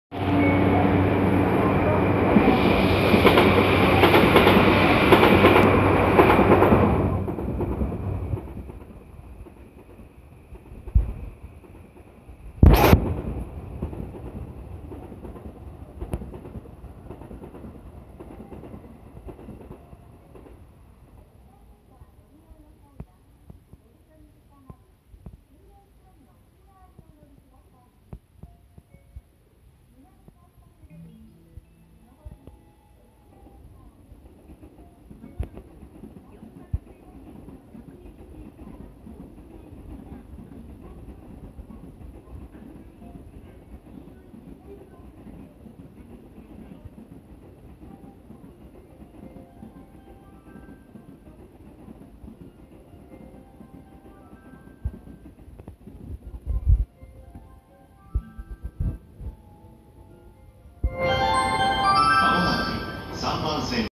田町駅前に立つムスブ田町前を通過していく東海道新幹線N700系の上下線
田町駅前に立つムスブ田町前を通過していく東海道新幹線N700系の上下線です。
田町駅前には、近年に完成した高層オフィスビルのツインタワーとなるムスブ田町がそびえ立っており、すぐそばの東海道新幹線の軌道をN700系の上下線が通過していきます。